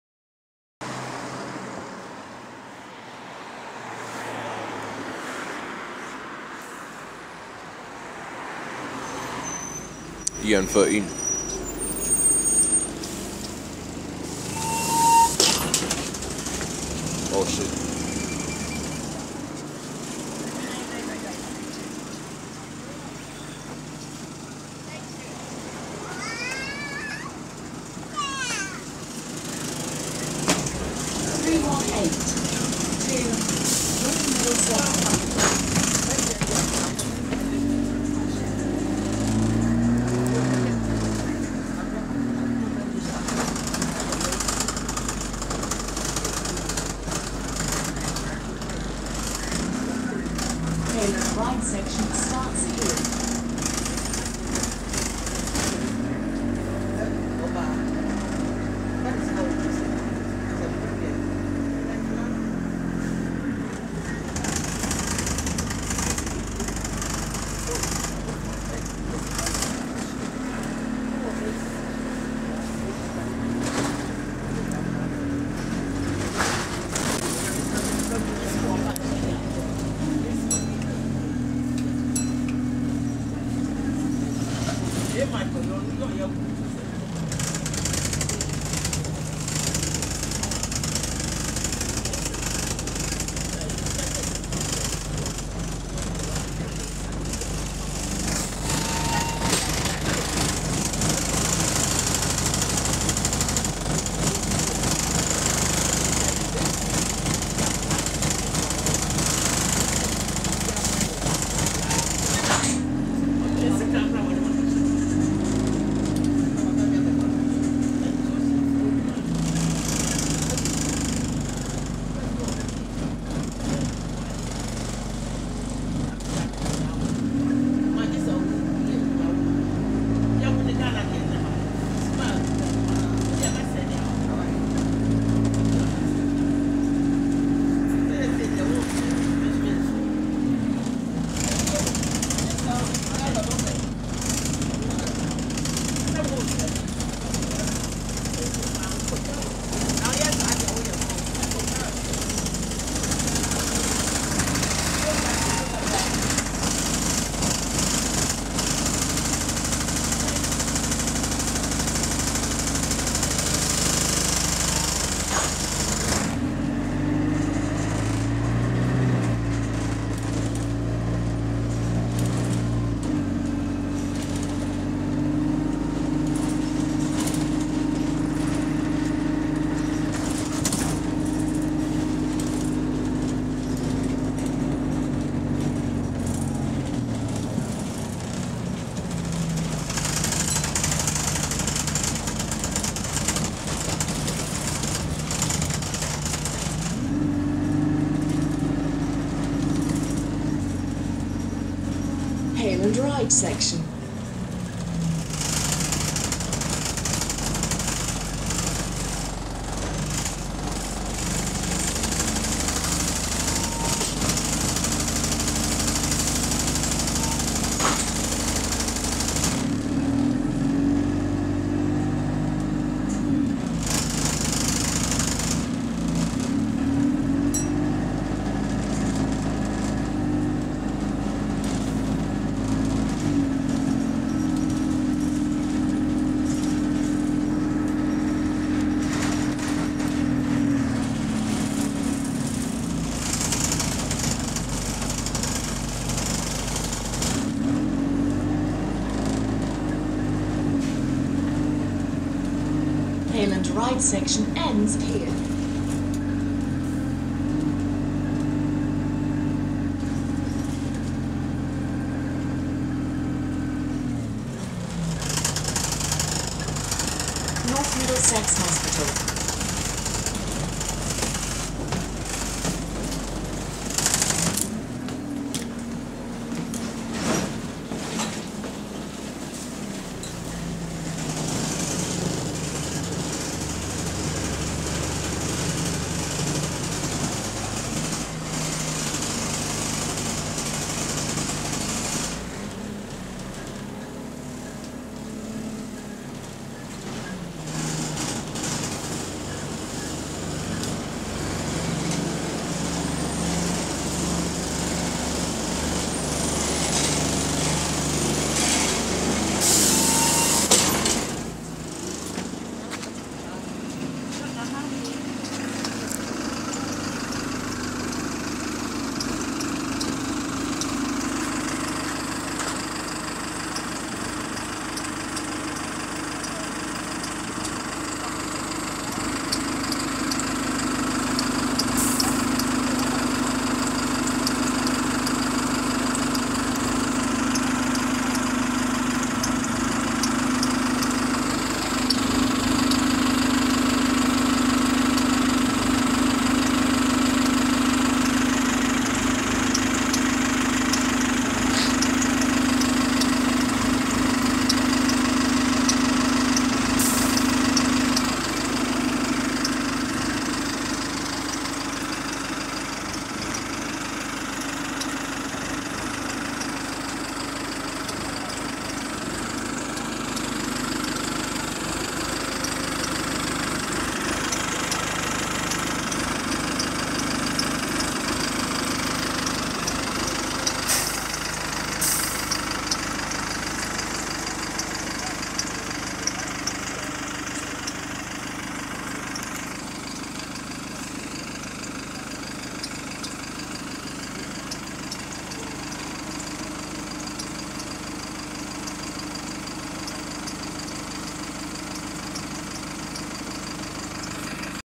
Choir of Parts Rattling to Idle, Jerky Idle | AL EN13 On Bus Route 318 (7)
brooo Arriva know how to handle e200s idc what nobody says they handle them the most characteristic way